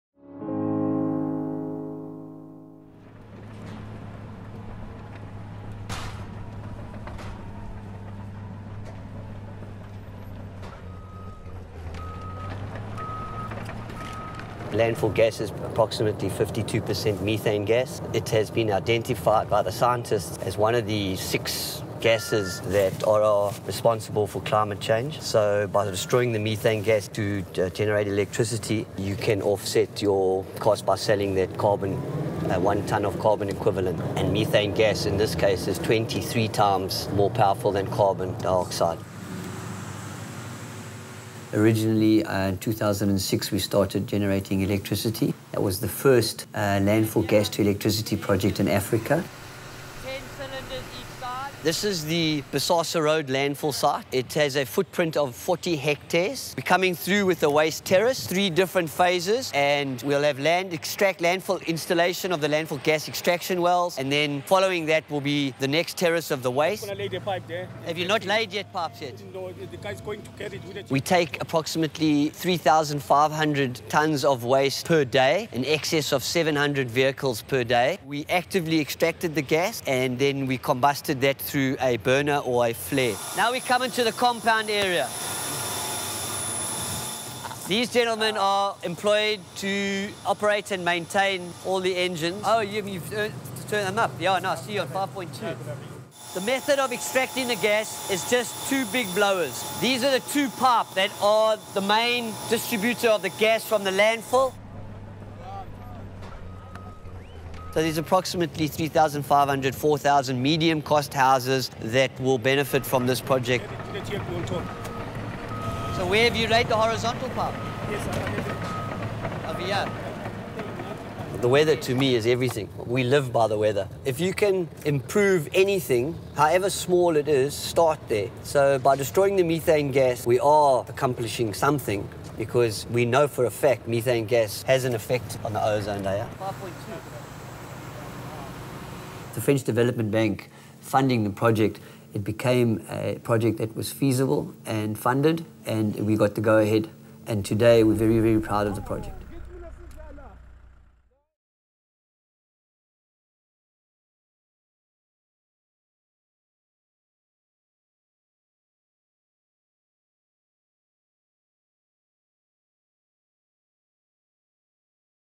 Afrique du Sud : de l’énergie propre à partir de déchets
L’AFD finance la production d’énergie à partir du méthane émis par les déchets de la ville de Durban. Ce projet innovant réduit les émissions de gaz à effet de serre.